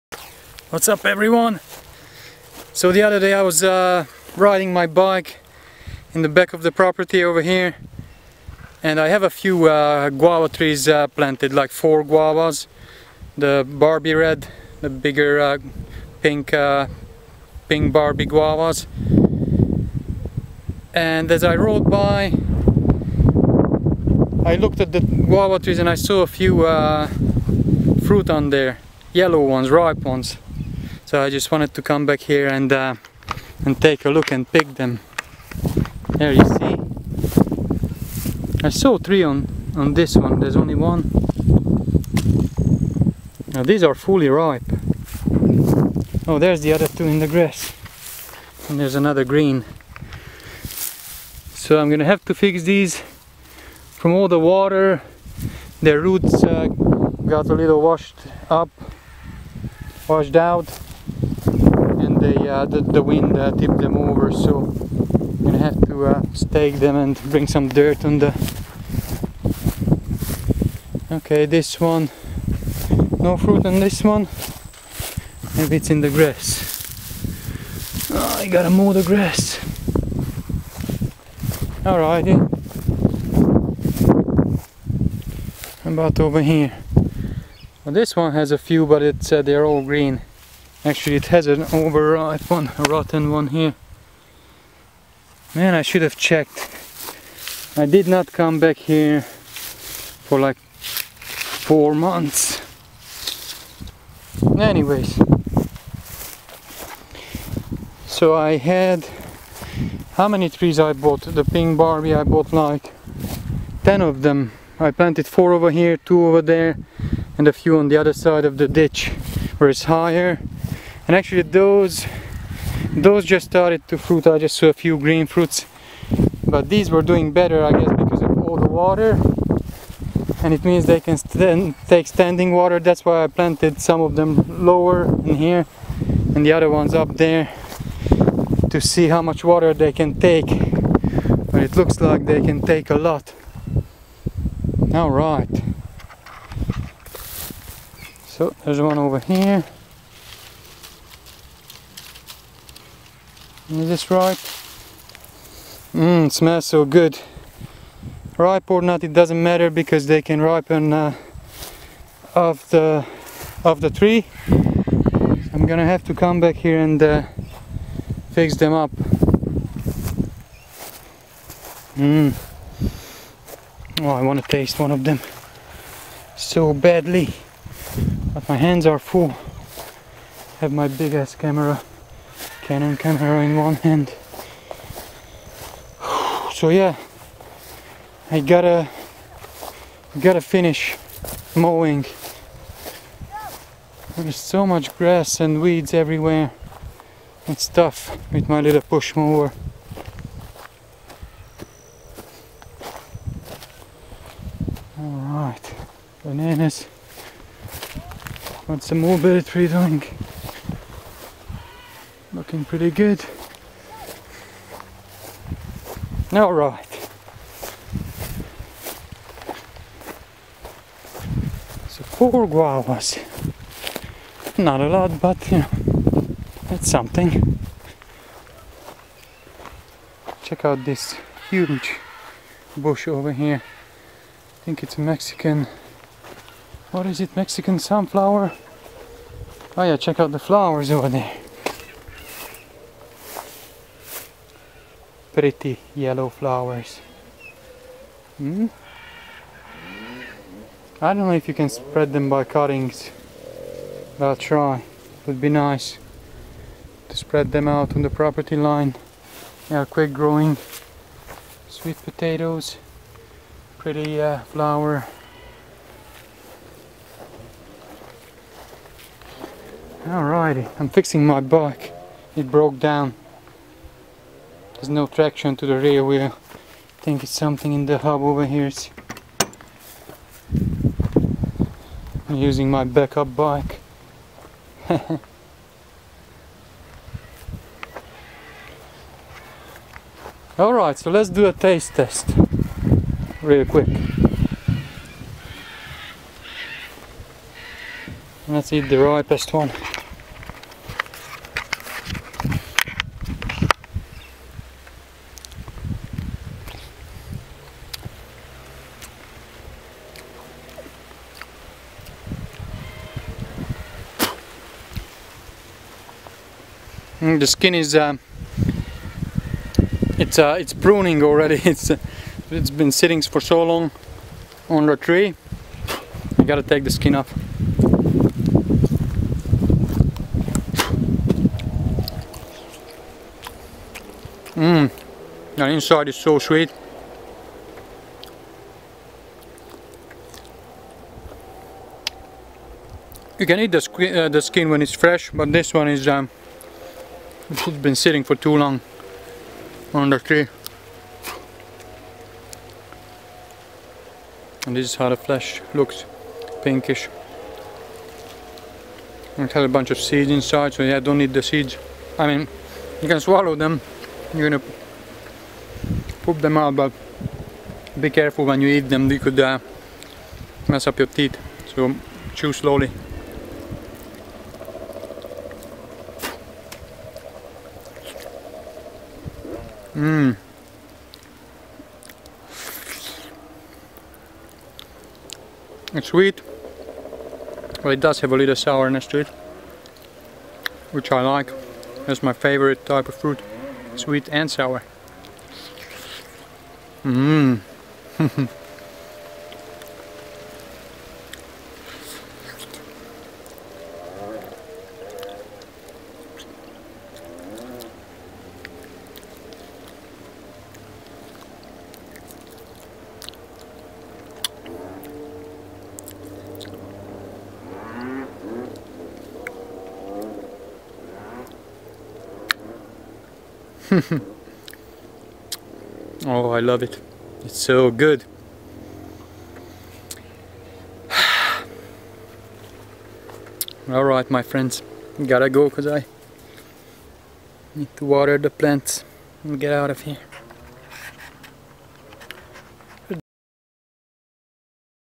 Picking and tasting my first guavas!
I forgot to adjust the camera focus.. sorry!

My 5 acre polyculture/permaculture food forest in the making in Naples, south Florida.